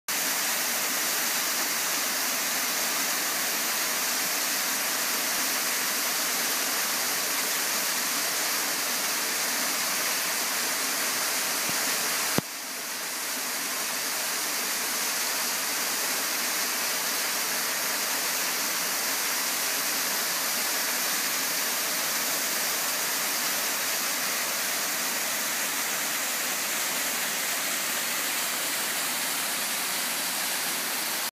Arenal Hanging Bridges Waterfall
This video was recorded in Arenal Hanging Bridges Park.